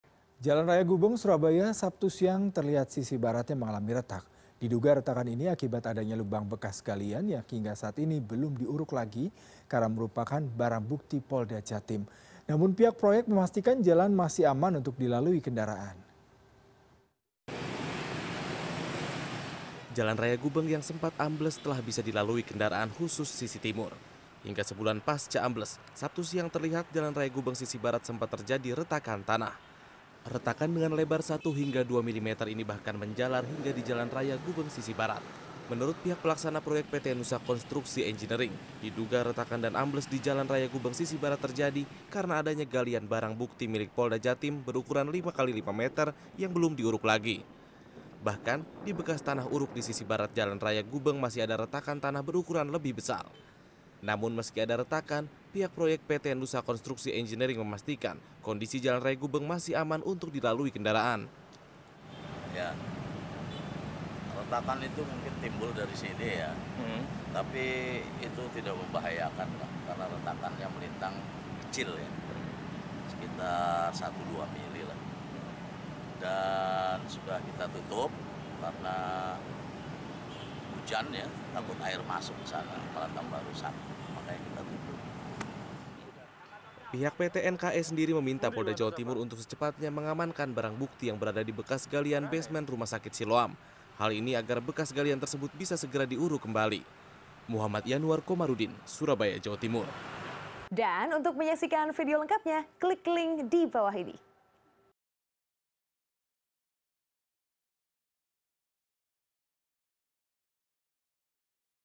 Jalan Raya Gubeng di Sisi Barat Retak
Jalan raya Gubeng Surabaya sabtu siang terlihat sisi baratnya mengalami retak. Diduga retakan ini akibat adanya lubang bekas galian yang hingga saat ini belum diuruk lagi, karena merupakan barang bukti Polda Jatim. Namun pihak proyek memastikan jalan masih aman untuk dilalui kendaraan.

Like & Follow Facebook:
CNN Indonesia

cnnindonesiatv